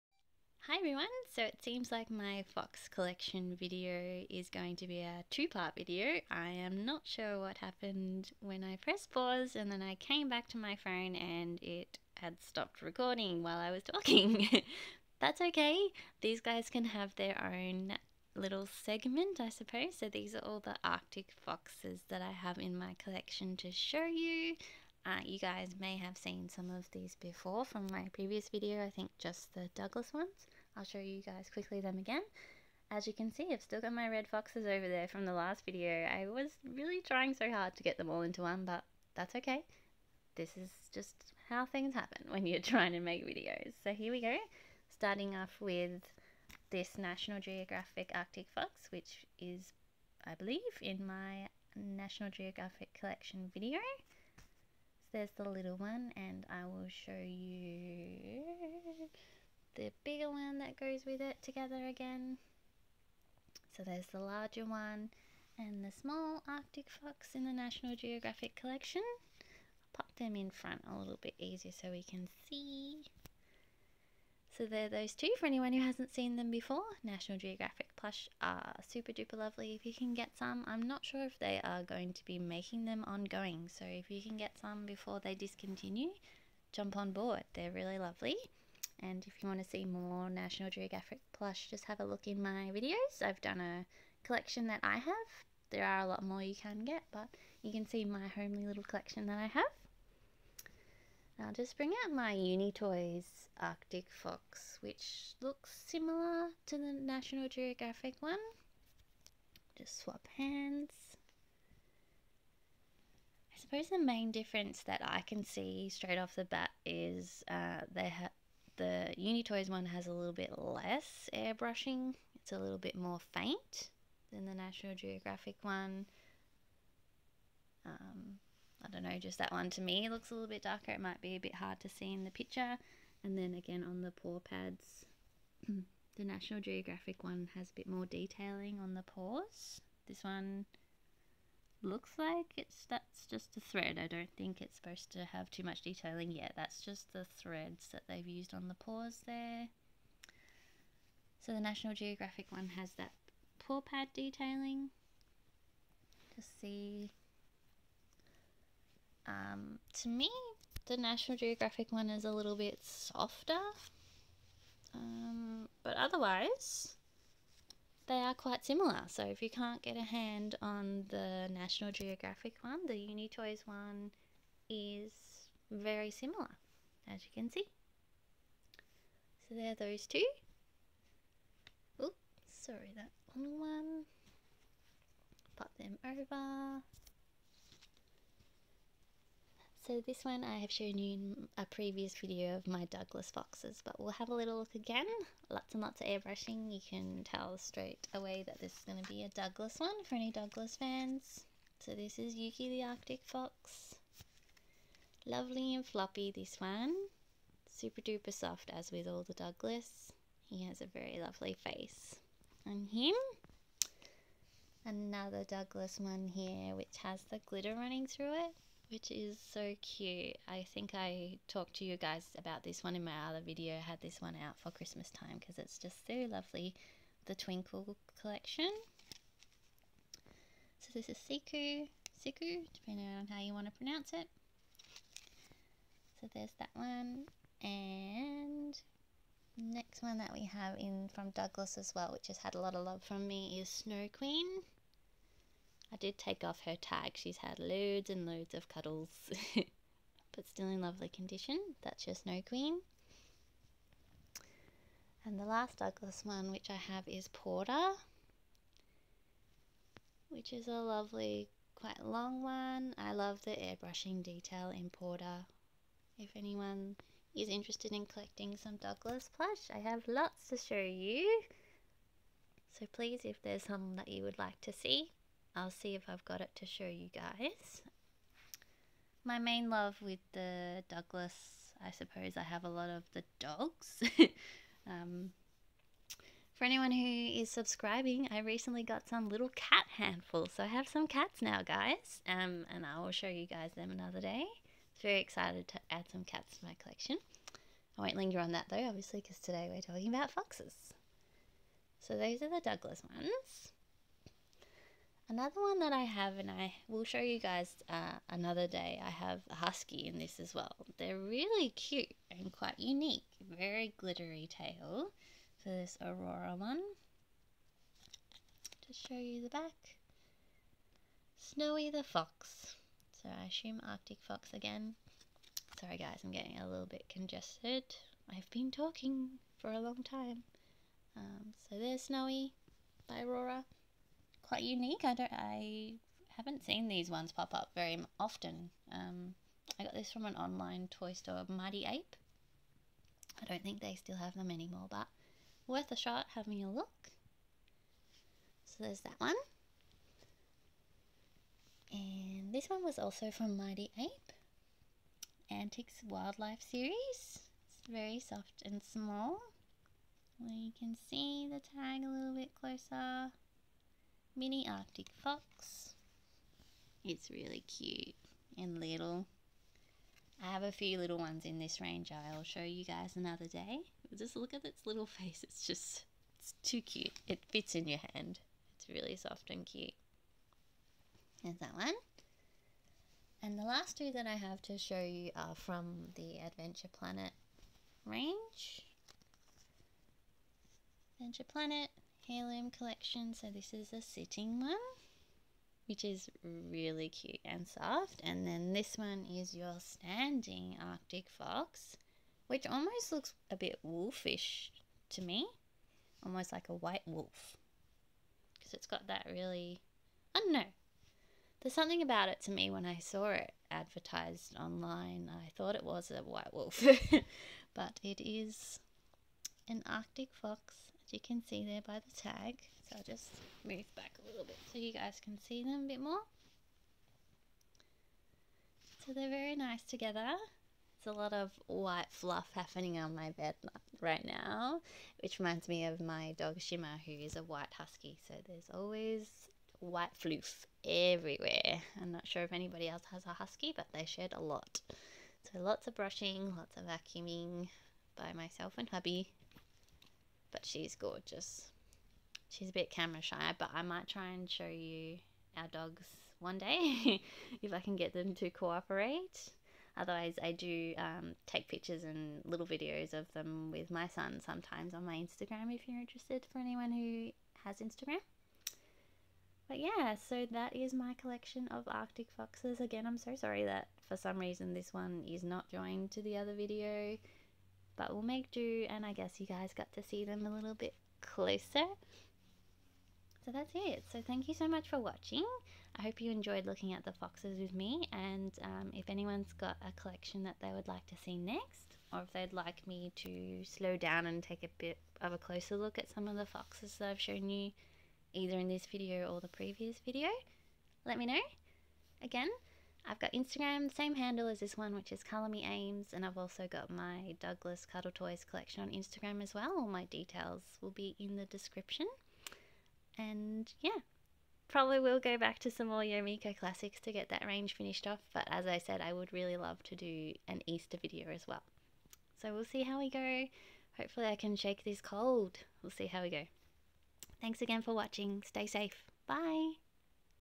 Plush Fox Collection Part 2: Arctic Foxes
Here is Part 2 of my Plush Fox Collection! I tried to get them all in one video but it didn't work out 😅 So please enjoy this second part which features my Arctic Foxes 🙂 Brands shown are Douglas, Adventure Planet, Uni-Toys, National Geographic, Aurora and Antics.

@colourmeaimz (main account)
@mydouglascuddletoys (Douglas plush only)